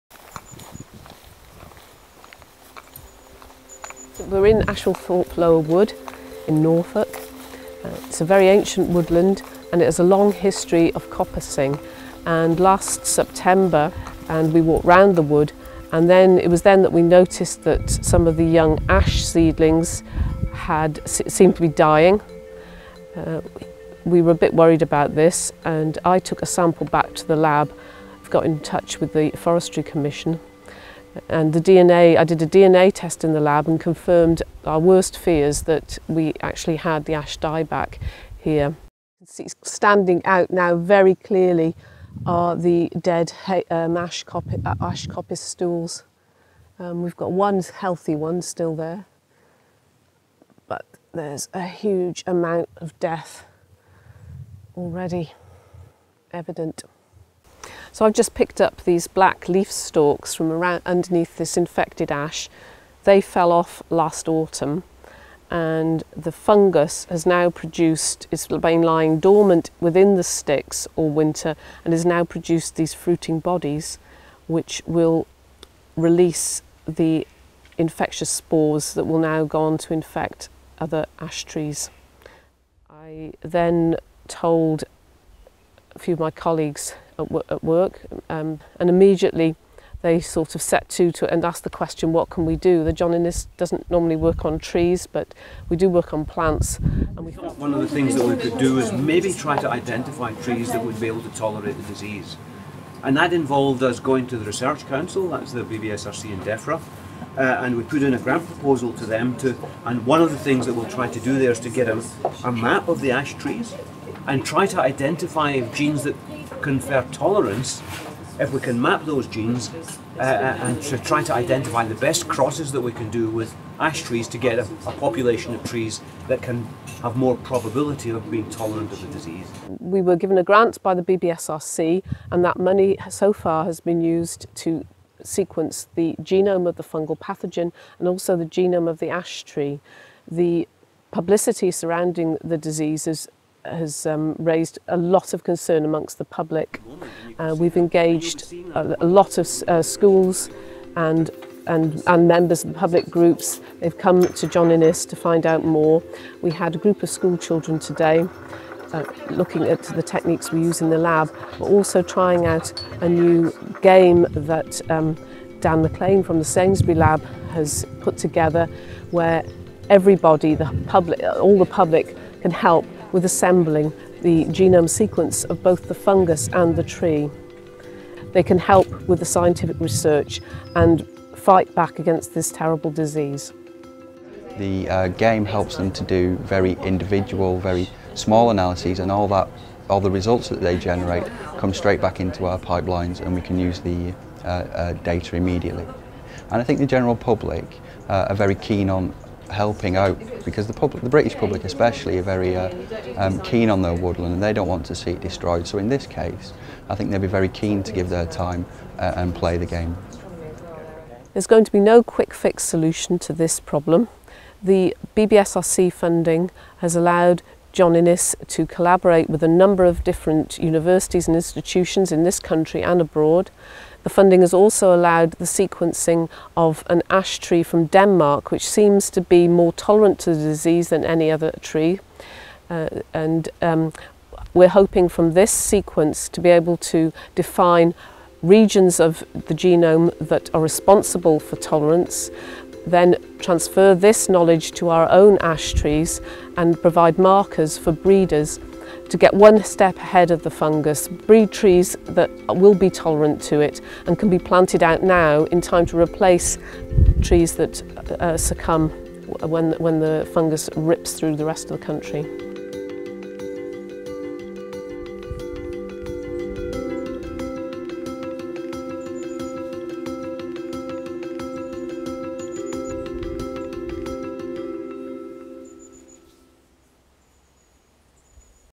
How We Found... And Are Fighting AshDieback Disease: A Threat to British
Ash dieback disease, a fungal disease that can devastate ash trees, was first discovered in the UK in 2012. With memories of Dutch elm disease still fresh in people's minds, this new threat became a significant concern to British woodland.

In this video, Dr Anne Edwards shares her experience finding and fighting this disease, which earned her a British Empire Medal (BEM) for her contributions to the environment and public understanding of science.

Join us to learn more about the threat of ash dieback and what we can do to protect Britain's 80 million ash trees.

Let’s connect!